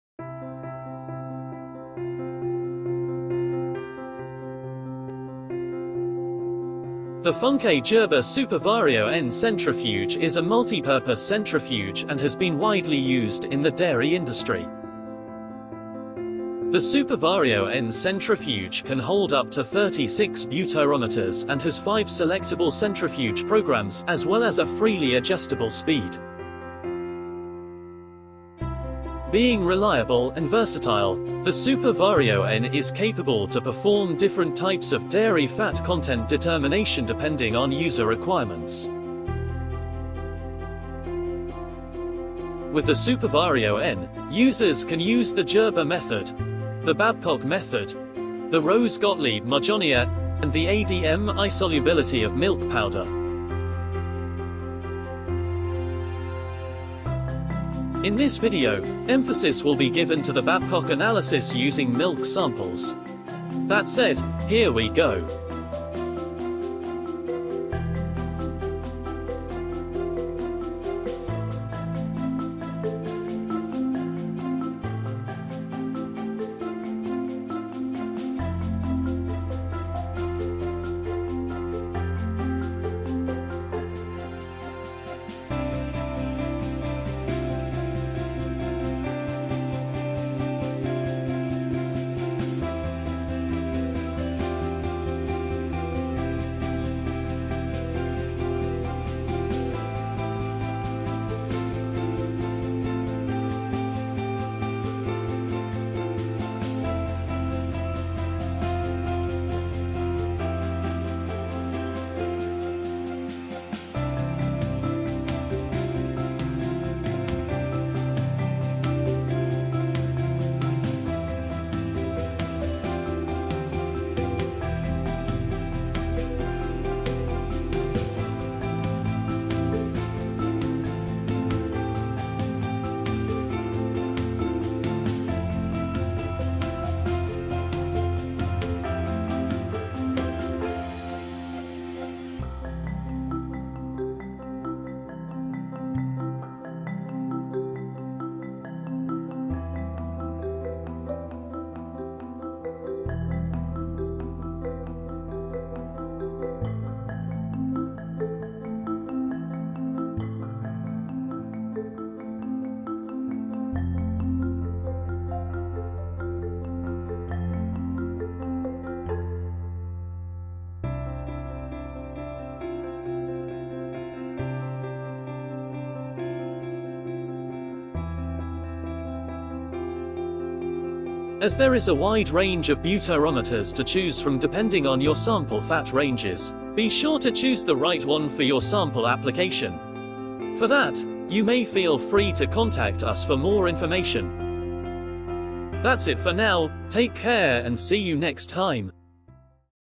Total Fat Analysis in Milk using the Babcock Method
Essentially this method of determining the percentage of butterfat in certain dairy products consists in transferring a definite weight of the product to be tested into a specially constructed and graduated test bottle. Sulphuric acid is the n added and mixed with the material in the test bottle. The bottles are then centrifuged or whirled three times in succession, at a definite speed and for a definite time. Hot water is added to the contents of the bottle after the first two whirlings. After the last whirling, the percentage of fat is read directly with a pair of dividers.